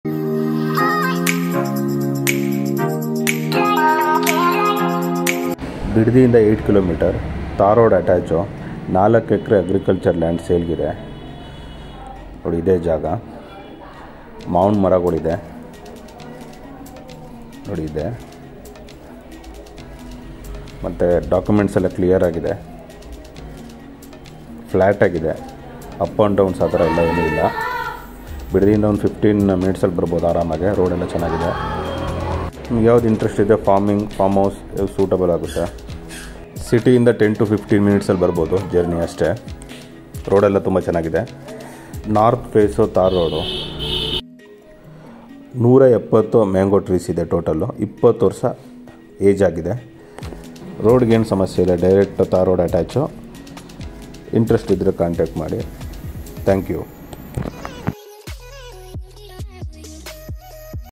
4 Acre Farm Land for sale in Bidadi | 2.5L/Gunta, Mob:7338474634, 8km from bidadi 10 minutes take
170 mango trees
20 years old
square shape Land
no Borwell
no fence
Tar road attached